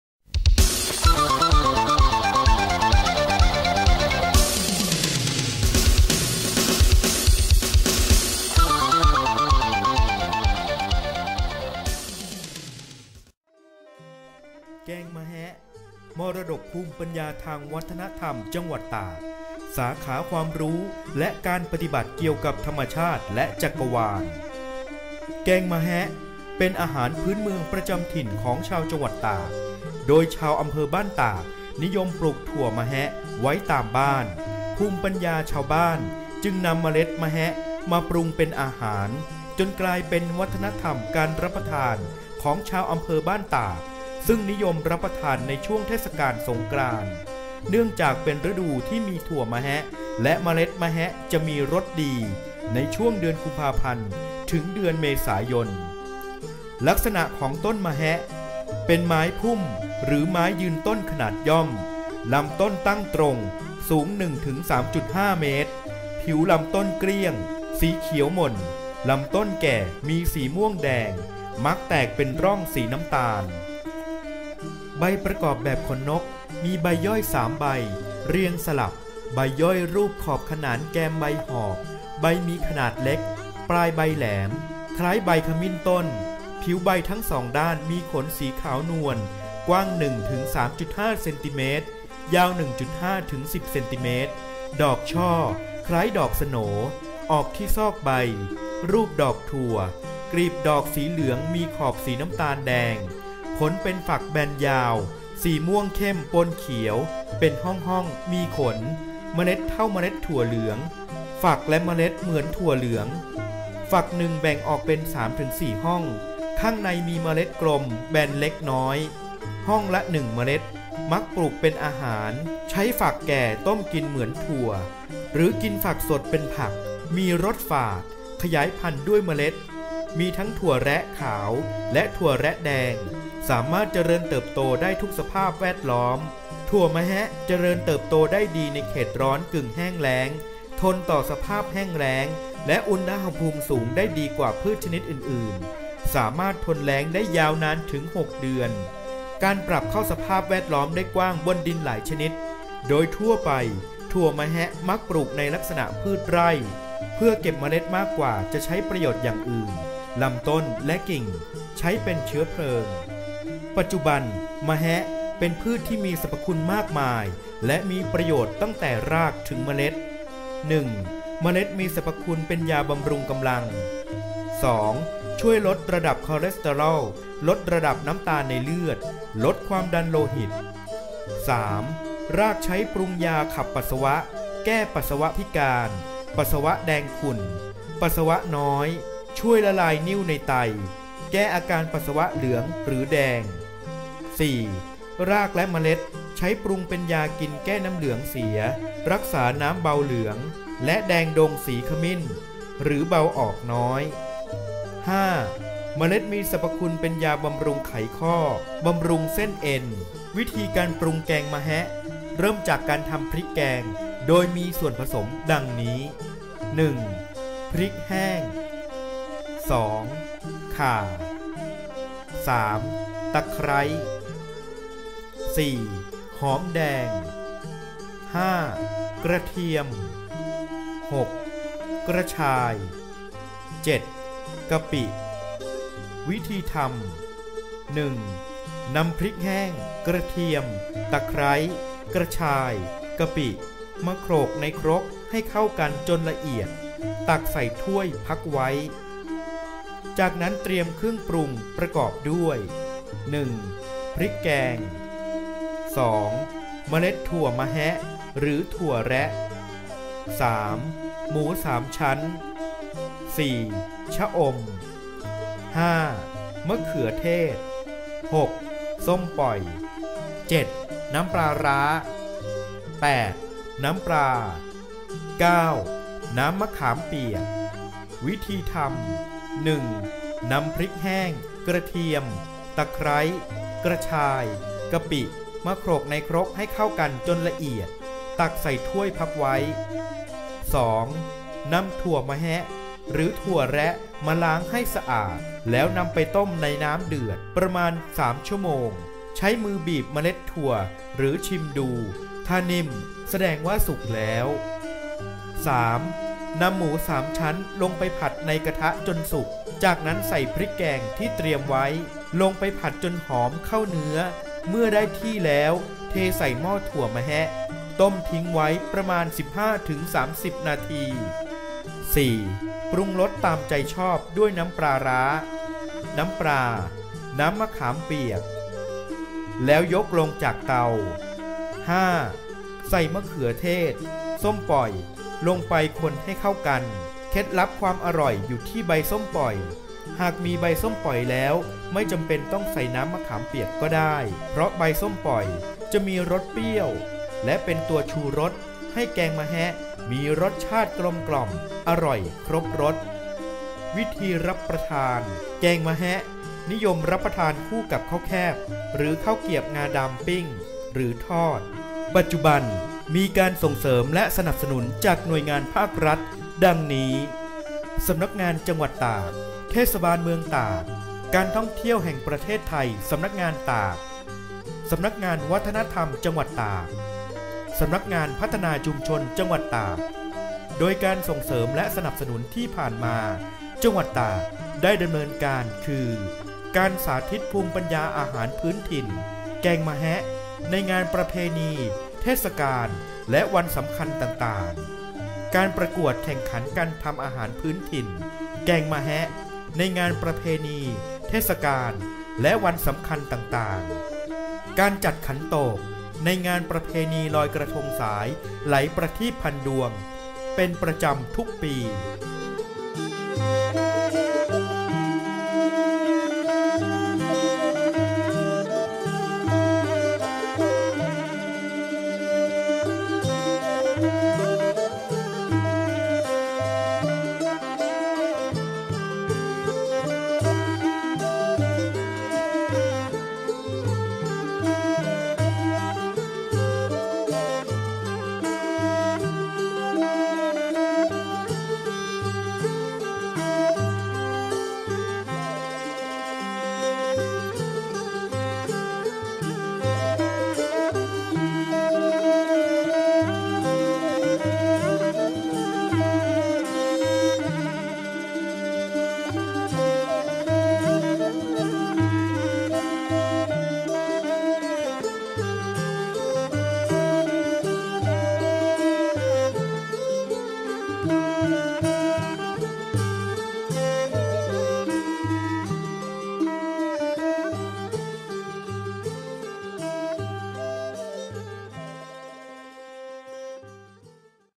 แกงมะแฮะ
มรดกภูมิปัญญาทางวัฒนธรรม จังหวัดตาก
สาขาความรู้และการปฎิบัติเกี่ยวกับธรรมชาติและจักรวาล

โดยสำนักงานวัฒนธรรมจังหวัดตาก กระทรวงวัฒนธรรม